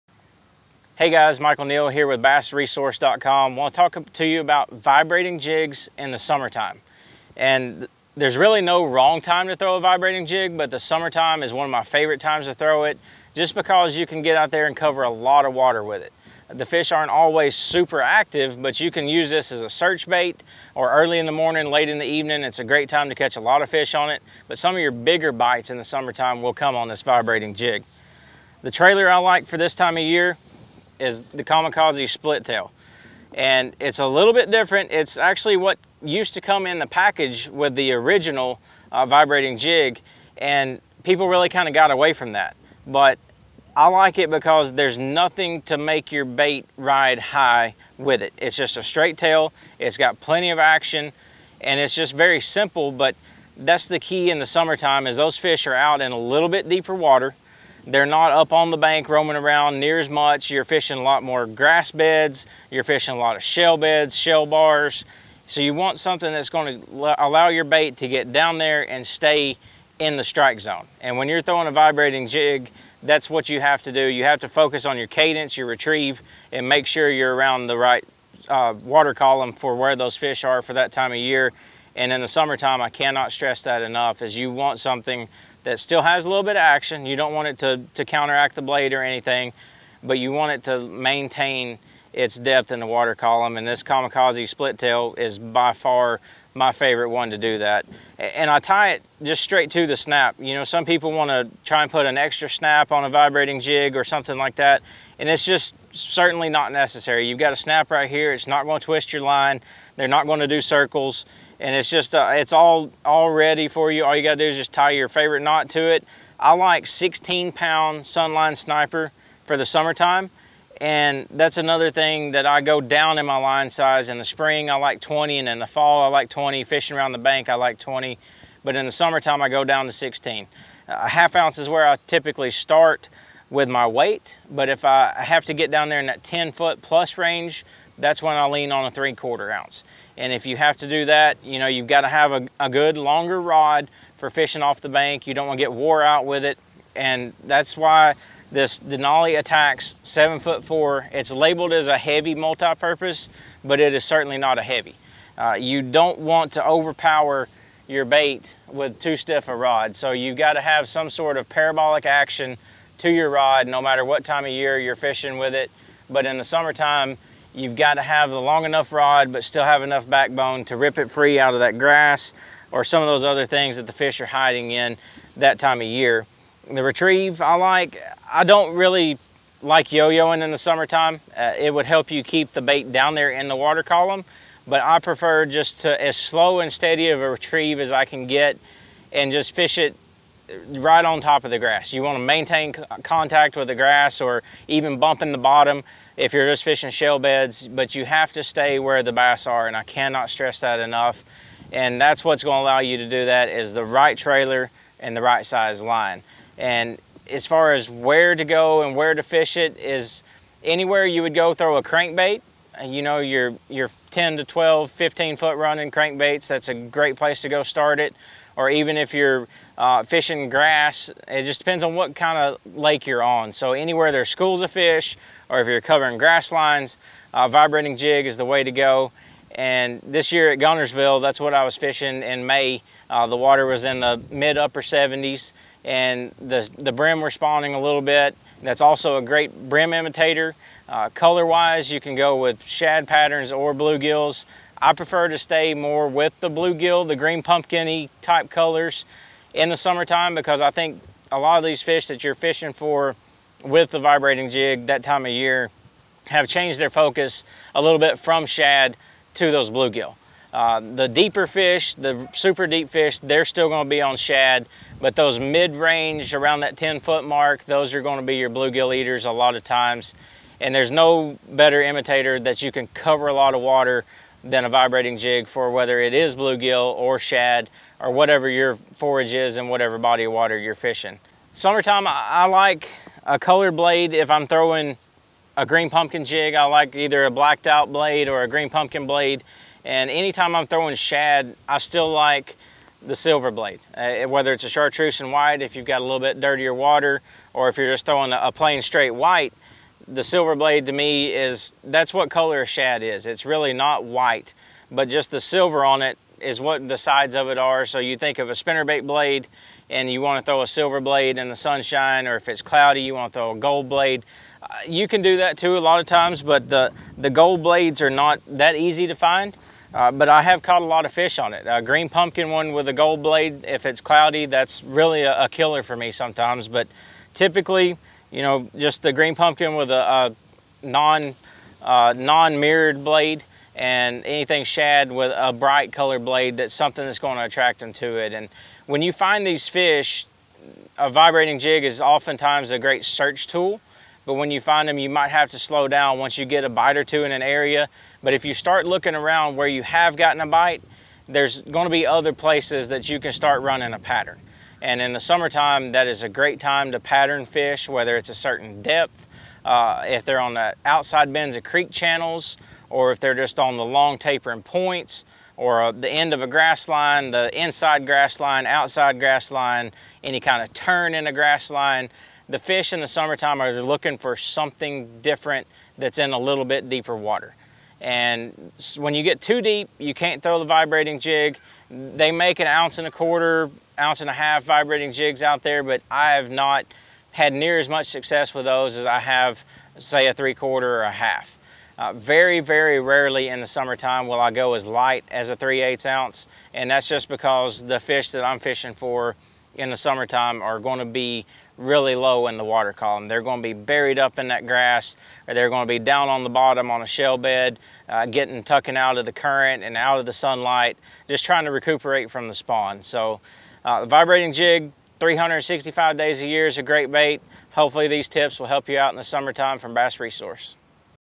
How To Fish A Chatterbait In The Summer | Bass Fishing
Learn summer Chatterbait secrets from 2-Time MLF Angler of the Year Michael Neal! Discover why bass love vibrating jigs in the spring!